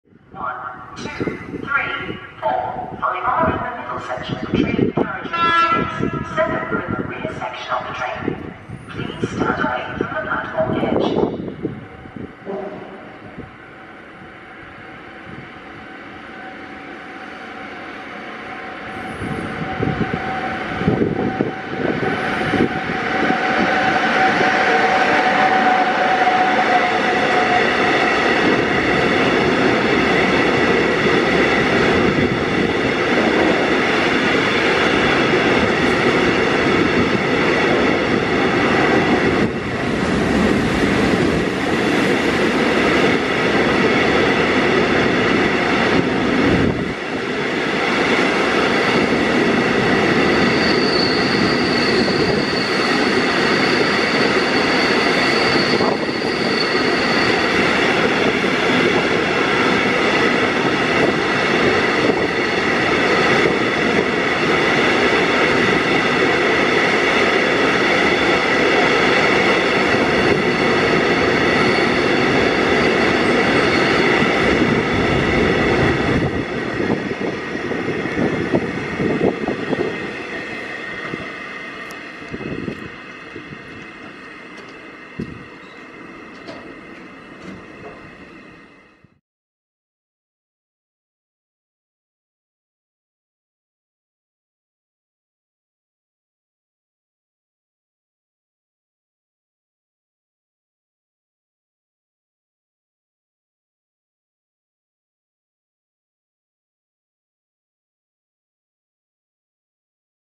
DRAGON Freightlinera w zimowej aurze! E6ACTd-101
12 marca 2023
Lublin Główny

Pierwsza z kolei maszyna tego typu w posiadaniu FPLa zmierza na wschód mijając pociąg z kijowa stojący przy sąsiednim peronie.